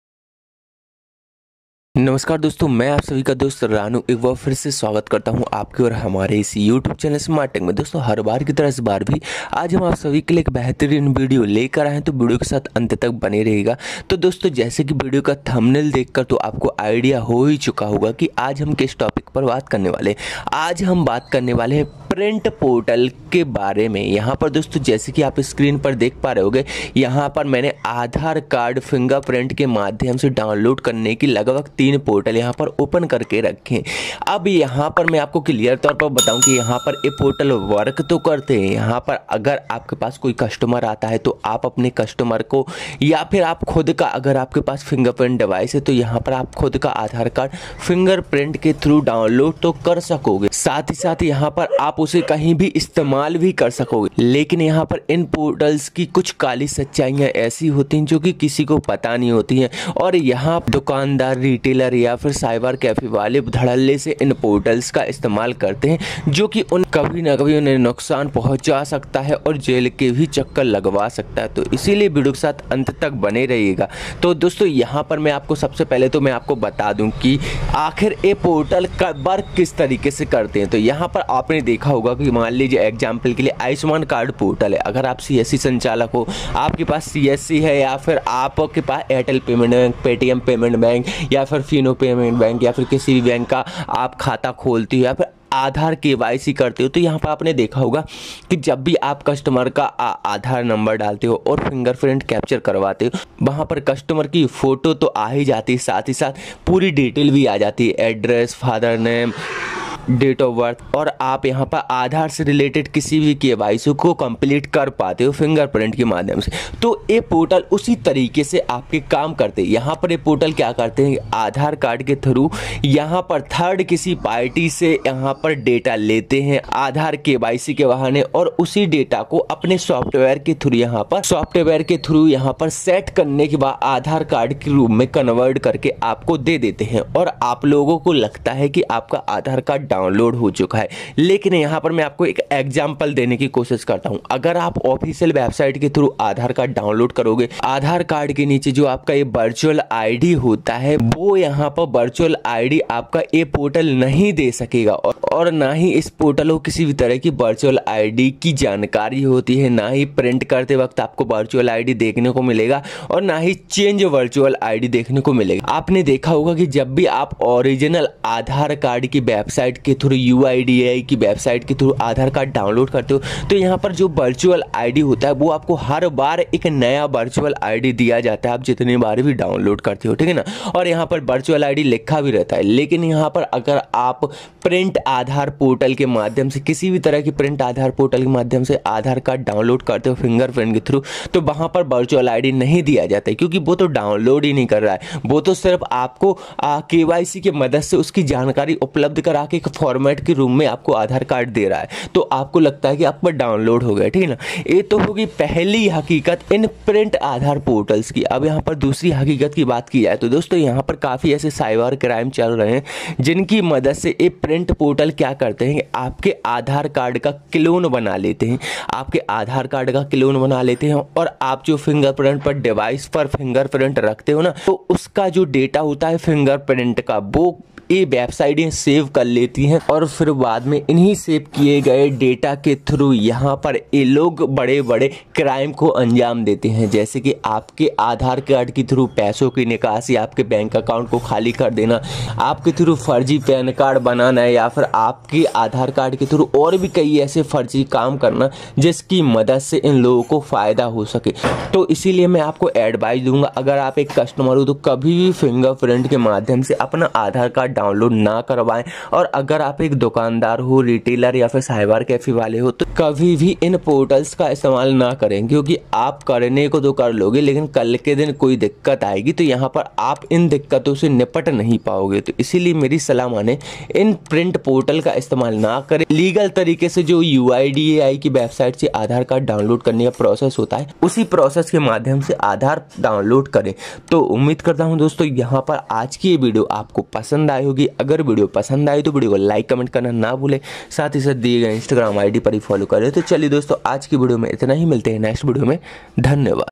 Print Portal Review - Printing Portal Real or Fake? | How print portal generate fake documents
dosto agr aap bhi print portals ki kali sachchai janna chahte he to aaj ki is video ko poora jarur dekhe dhanyawaad.